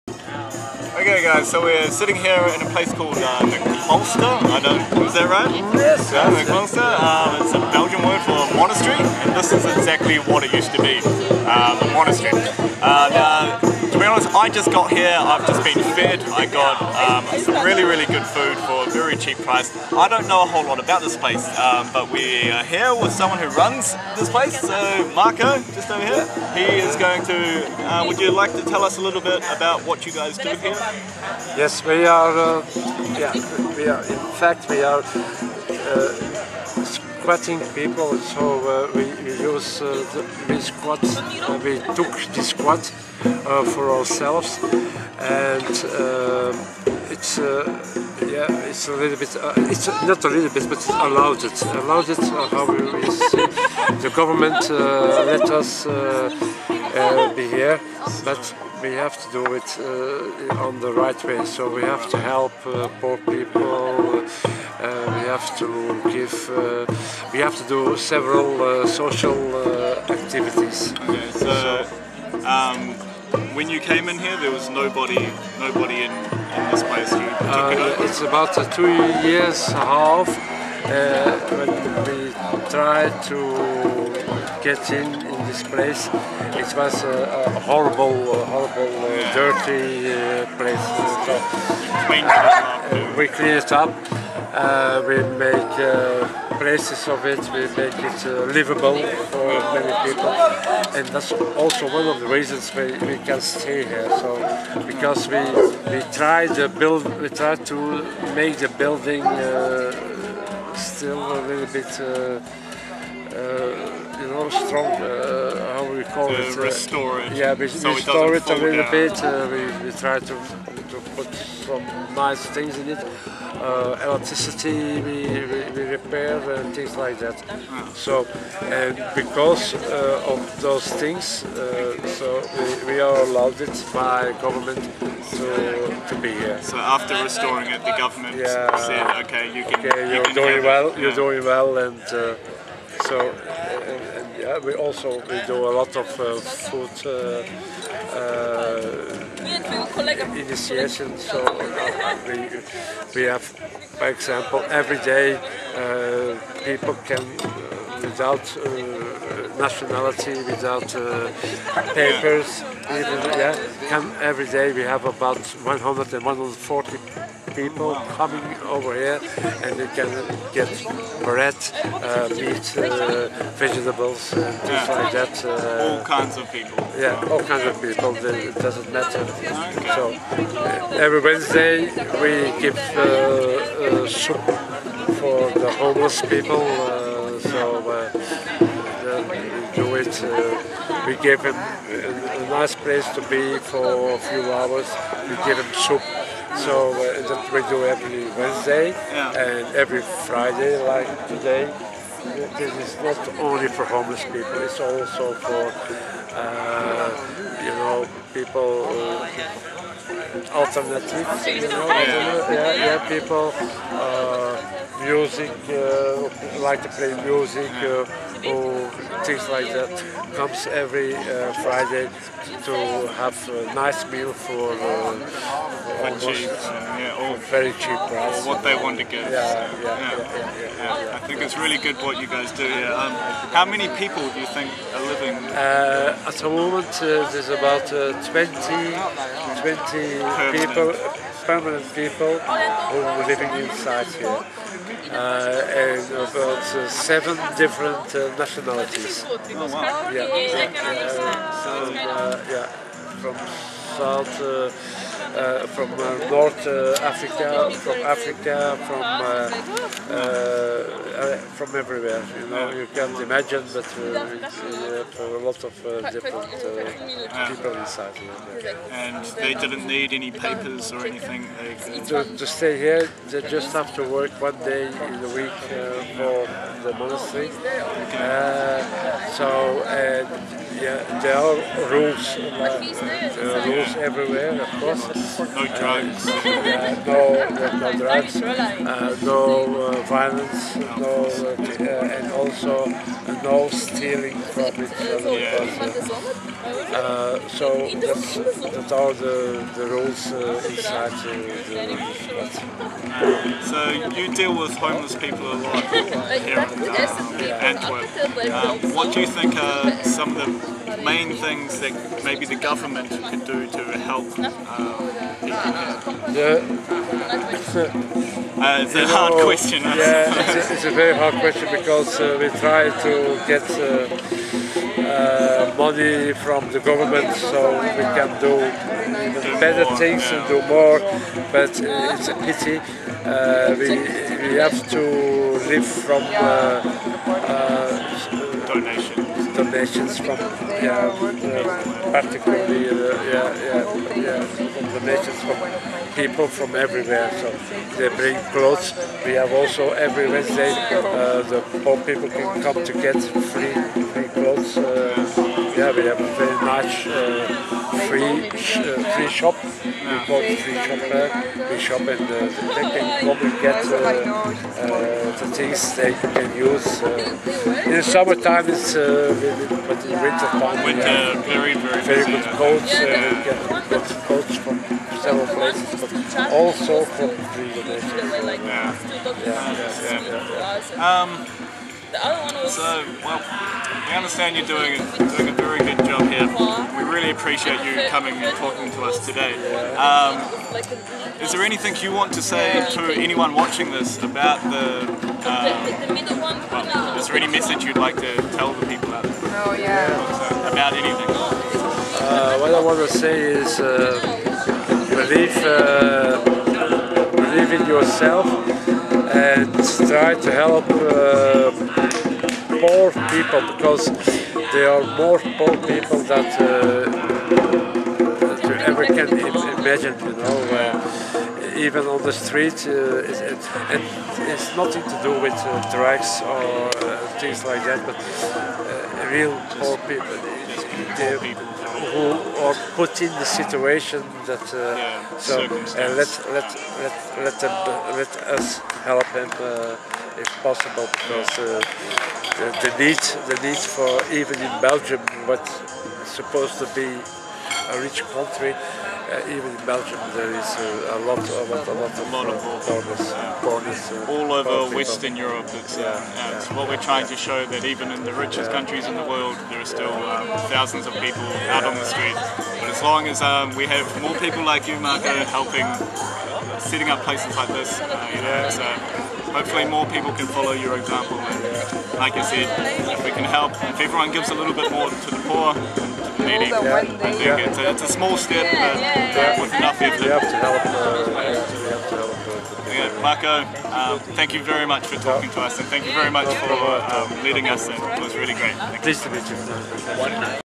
Marco and the Antwerp squat. Working to help the poor
This is a short introduction to Marco. We got a lot more footage of him talking, which will be cut into our final documentary.